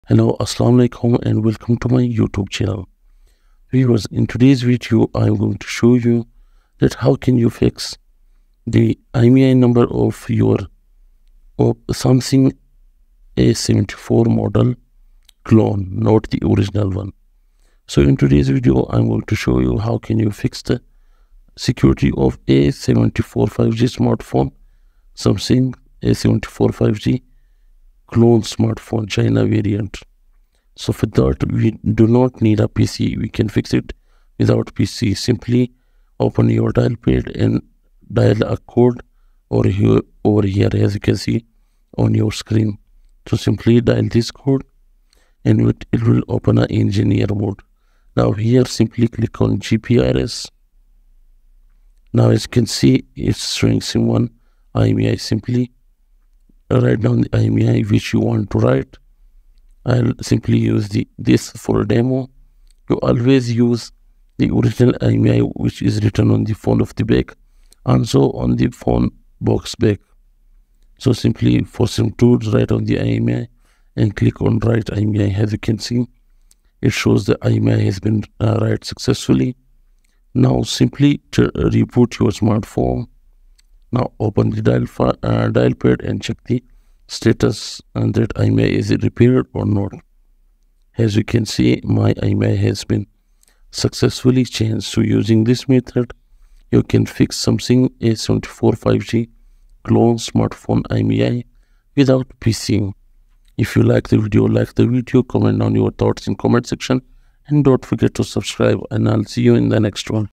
Samsung A74 5G IMEI Repair Without PC | Security Fix | A74 5G Clone Fix 2024
"Learn how to repair IMEI on Samsung A74 5G without PC! In this tutorial, we'll show you the step-by-step process to fix IMEI issues on your Samsung A74 5G, including clone devices. Follow our easy guide and restore your device's original IMEI in no time!

In this video, we'll cover:

- Samsung A74 5G IMEI repair without PC
- Security fix for IMEI issues
- Fix for clone devices
- Step-by-step tutorial for easy IMEI repair

No technical expertise required! Our easy-to-follow instructions will help you repair your Samsung A74 5G's IMEI in no time.